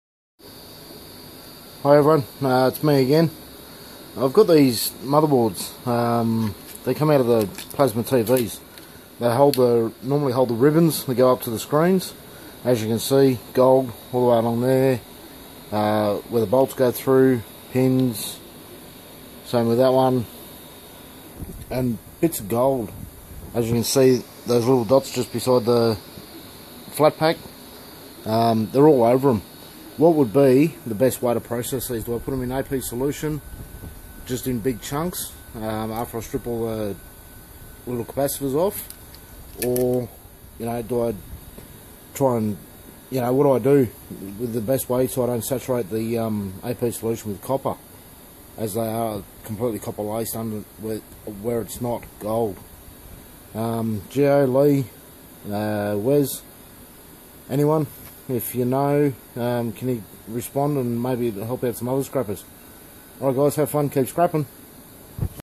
Gold Recovery plasma TV gold
gold laced mother boards out of plasma tv or led, lcd tv need some help with recovering gold